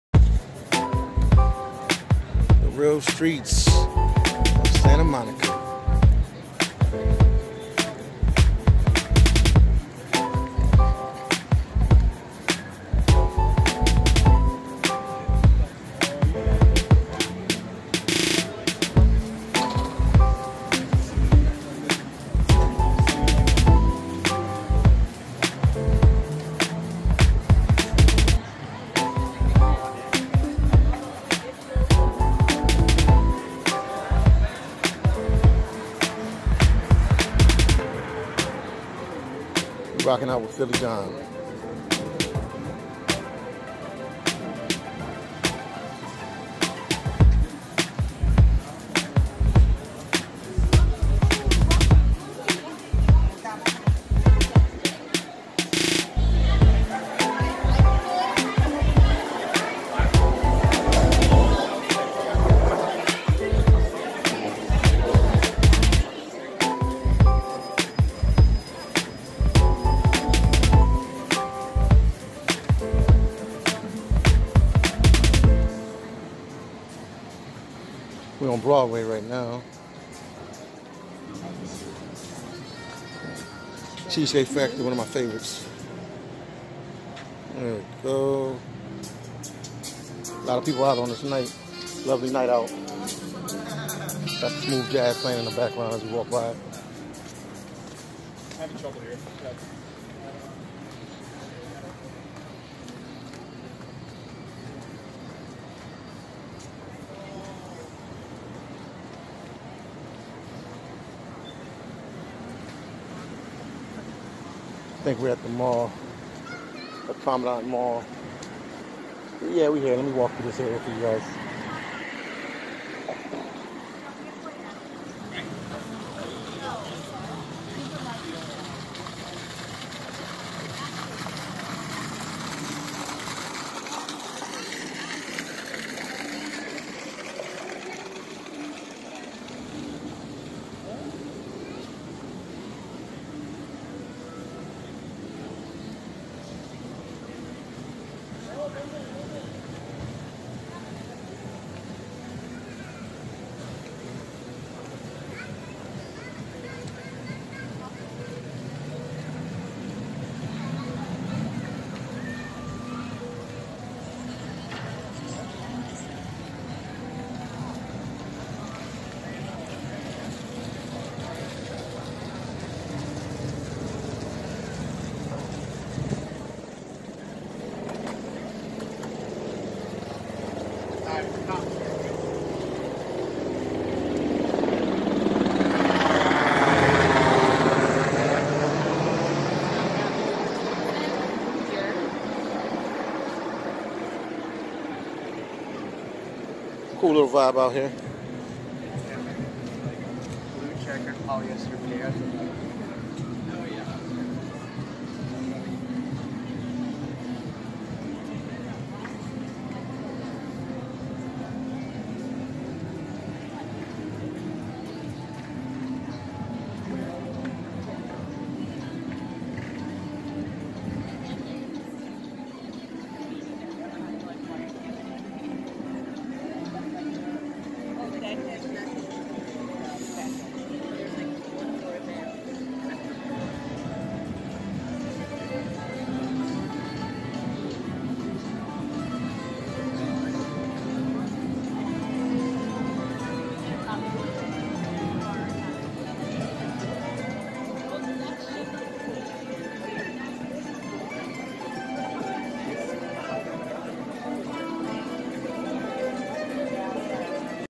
THE REAL STREETS OF SANTA MONICA CALIFORNIA 2019 (PART 2) || NIGHTLIFE
Walking around SANTA MONICA CALIFORNIA REAL STREETS AND NIGHTLIFE AREA/LOS ANGELES CALIFORNIA

Hello, welcome to my Youtube Travel VLOG, I

FACEBOOK PAGE HERE: PHILLY DOM

I'M ALWAYS GRATEFUL YOU WATCHING MY VIDEOS..

ESPANOL AQUI